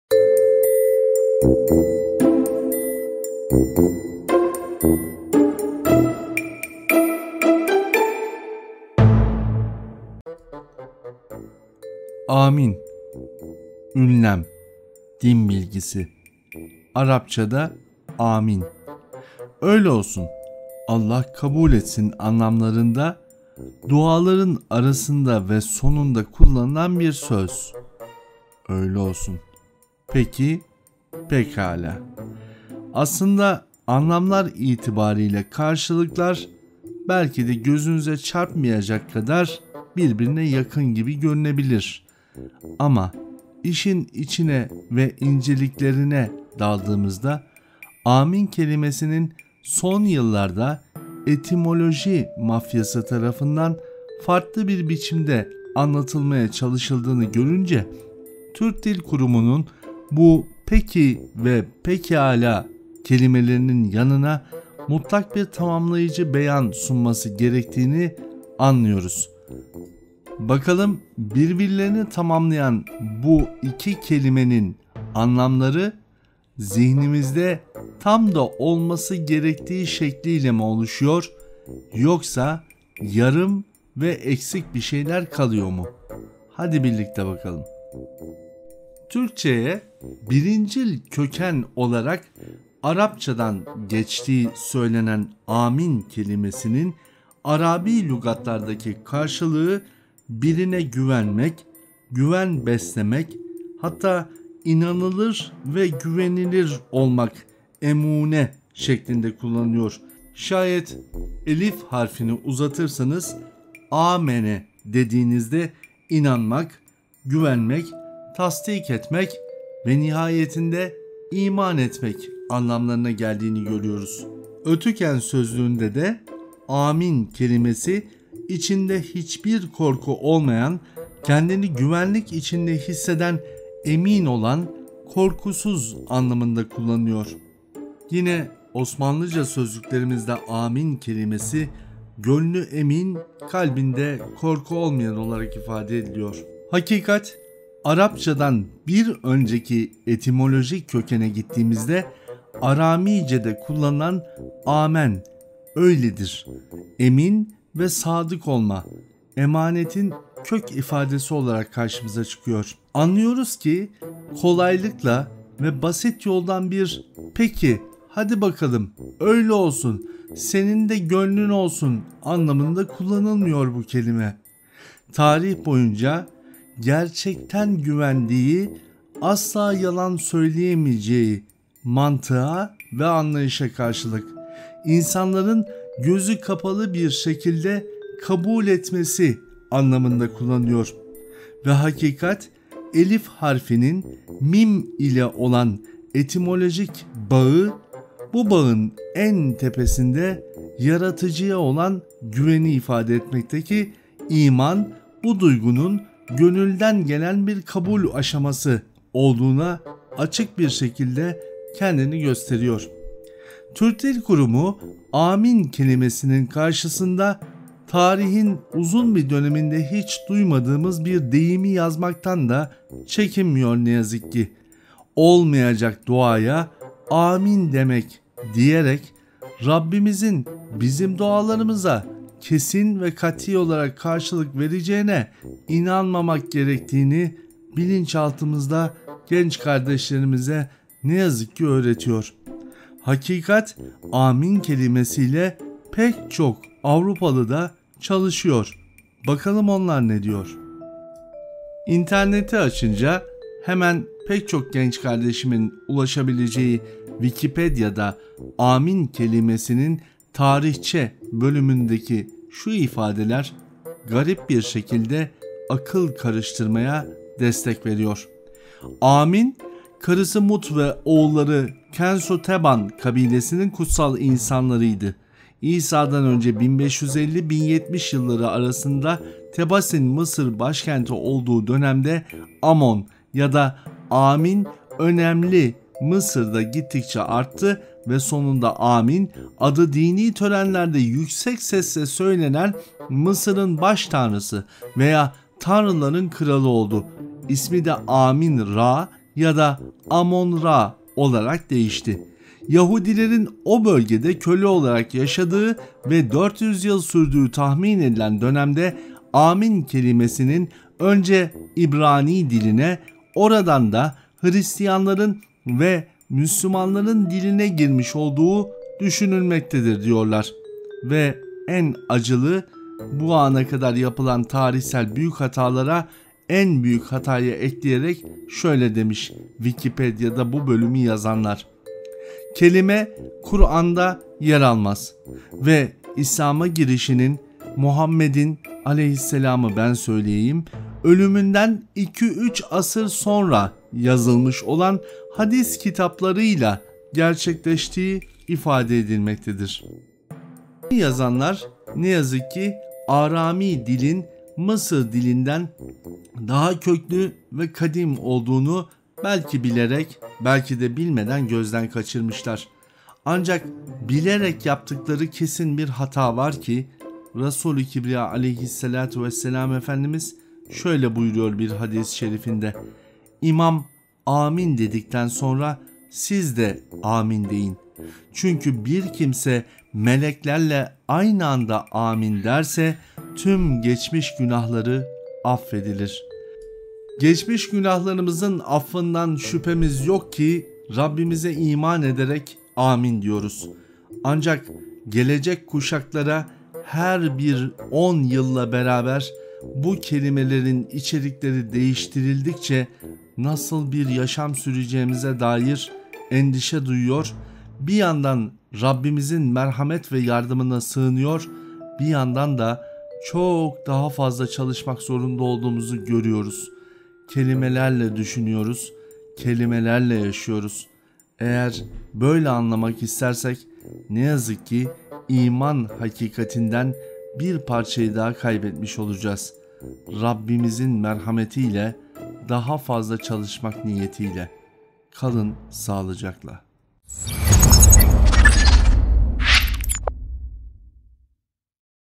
Amin; Ama Neden?
Söz Arası Bölüm 17

Amin deriz duaların kabulü için. Amenna deriz onaylamak için. Bir kabulleniştir amin. Güven ve inancın belirtisidir.

Kimilerinin dediği gibi Amon'dan falan da gelmez. Etimolojisi Aramice'ye dayanır.

Neyse buyurun dostlar diyelim.